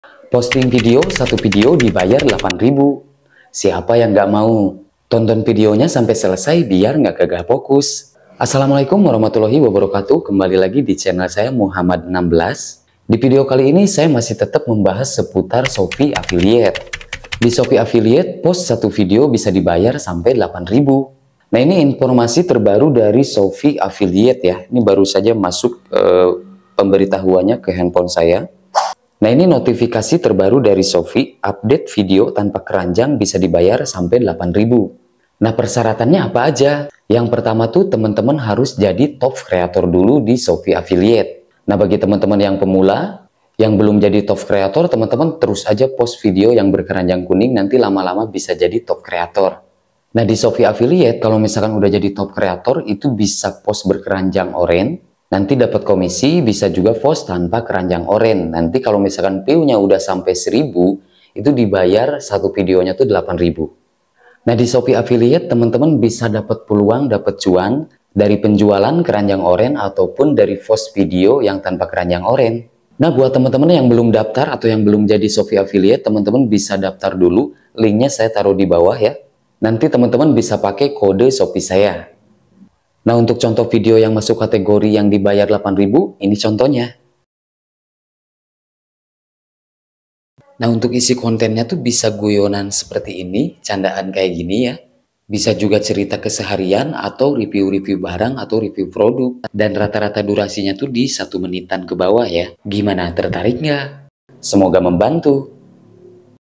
Post Video Di bayar 8000 | Peluang cuan baru di shope afiliet/Gass Mumpung belum banyak yang tau
Post video tanpa keranjang di bayar 8000 di shope afiliet
Peluang baru untuk dapat uang tambahan

Ada lertanyaan silahkan komentar